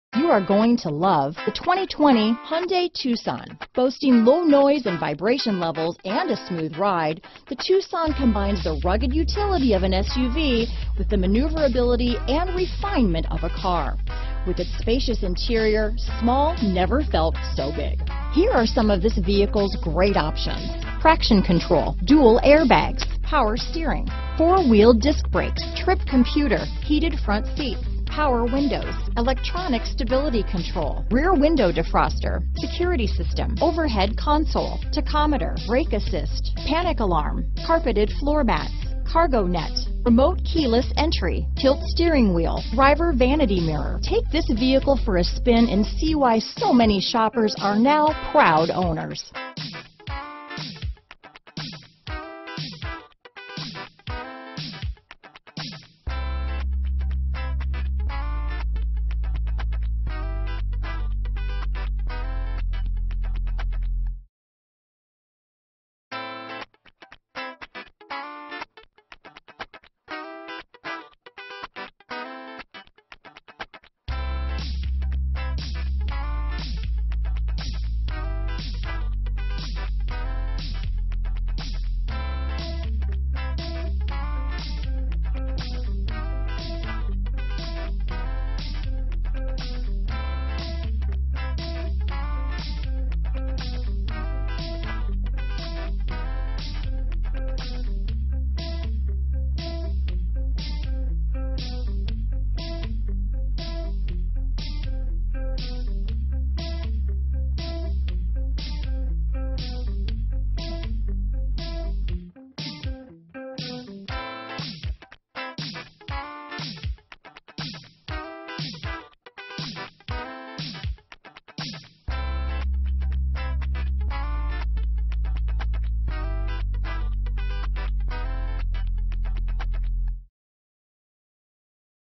2020 Hyundai Tucson Carson City, Lake Tahoe, NV HY939
Black Pearl [LISTING TYPE] 2020 Hyundai Tucson available in Carson City, Nevada at Carson Car Center. Servicing the Lake Tahoe, NV area.

2020 Hyundai Tucson SEL SEL AWD. 6-Speed Automatic with Shiftronicbrbr2.4L 4-Cylinder AWD 4D Sport Utility